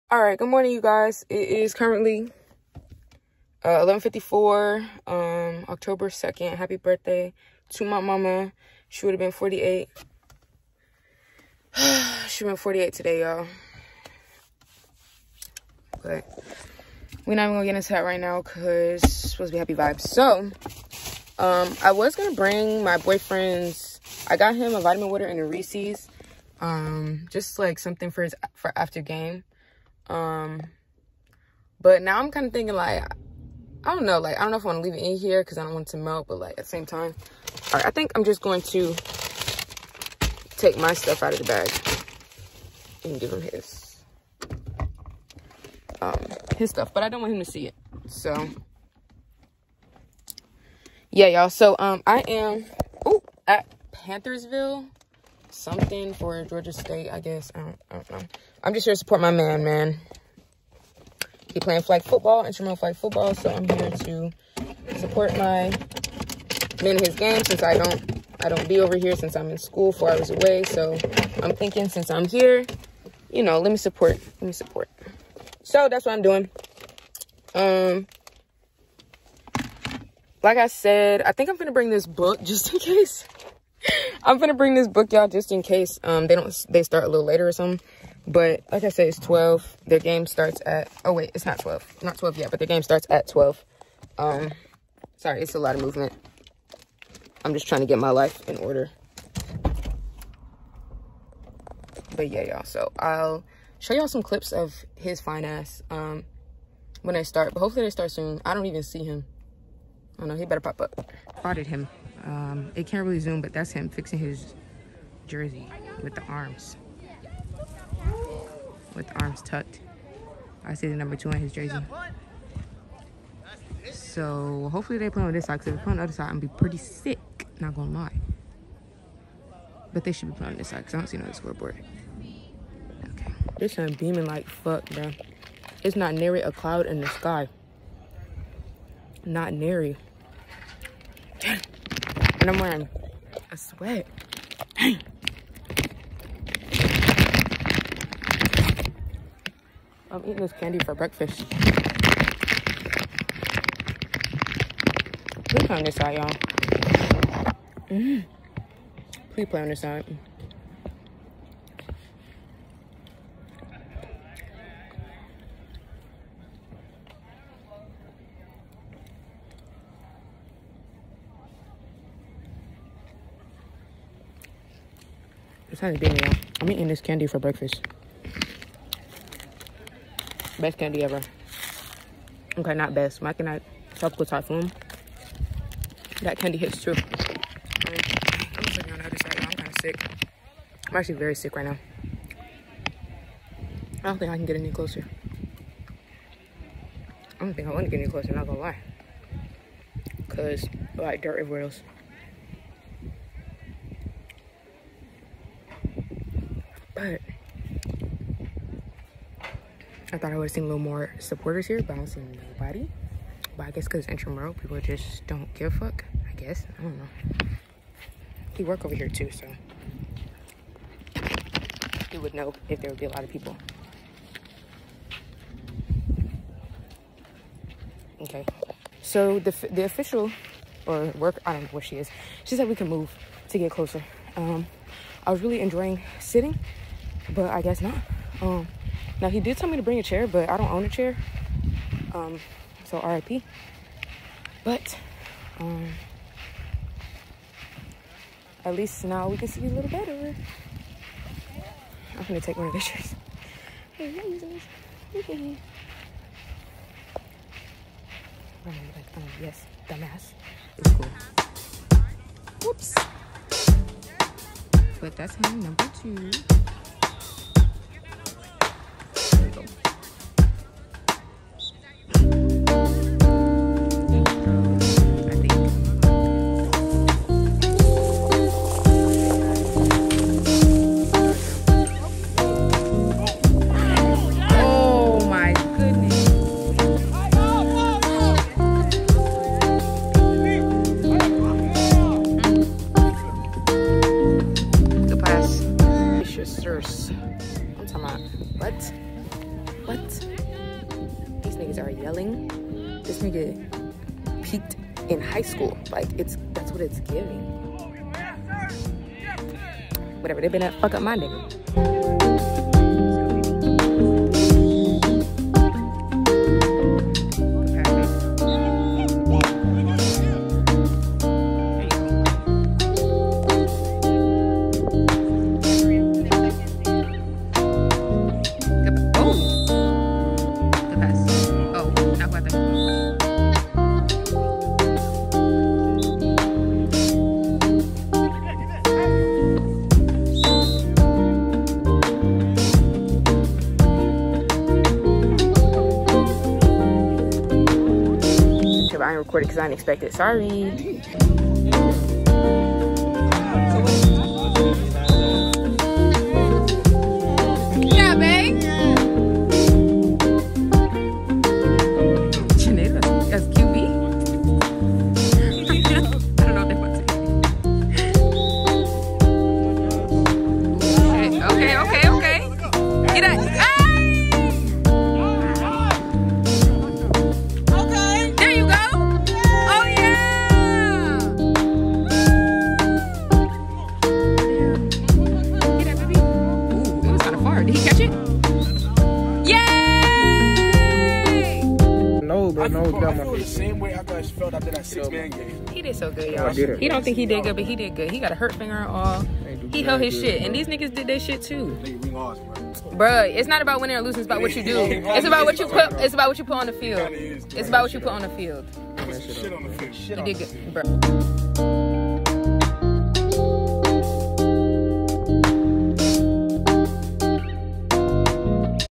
journal entry twenty nine: my boyfriend's flag football game
my last day home, I went to watch my boyfriend play

Tattoo Business: @creativesparkink

Happy Birthday, Mommy 💕